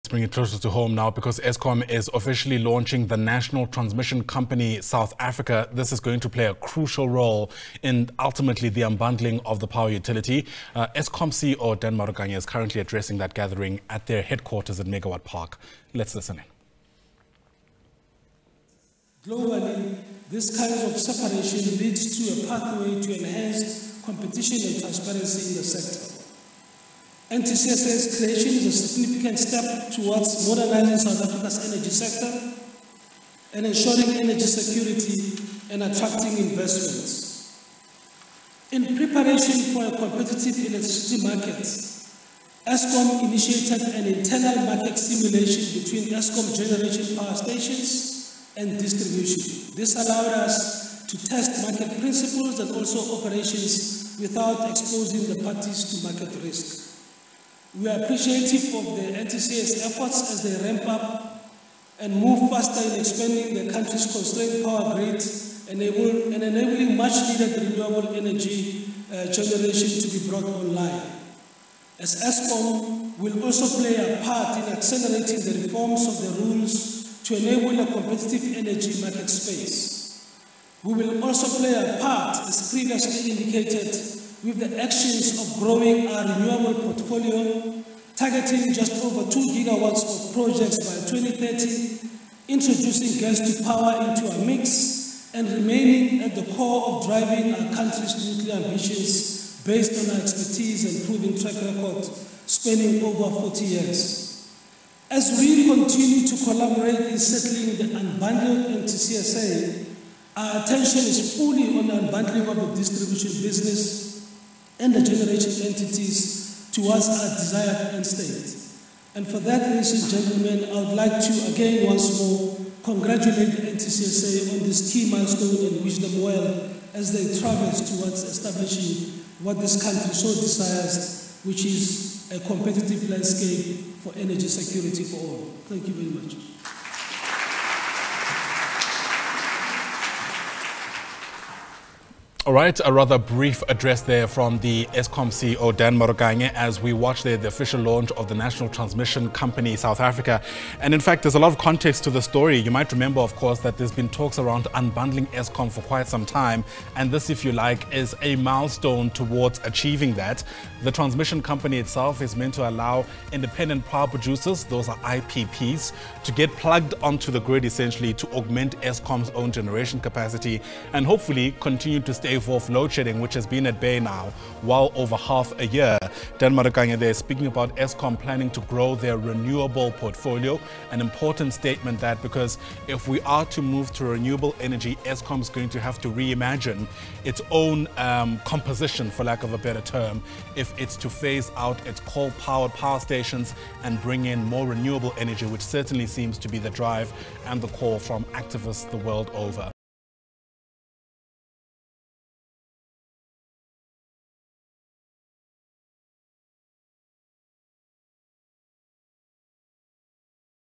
Eskom I The National Transmission Company South Africa and renewables portfolio: Dan Marokane
Eskom will today mark the official launch of the National Transmission Company South Africa.
NTCSA plays an important role in managing the transmission of electricity across the country.
For more, we are joined by our reporter Katlego Legodi.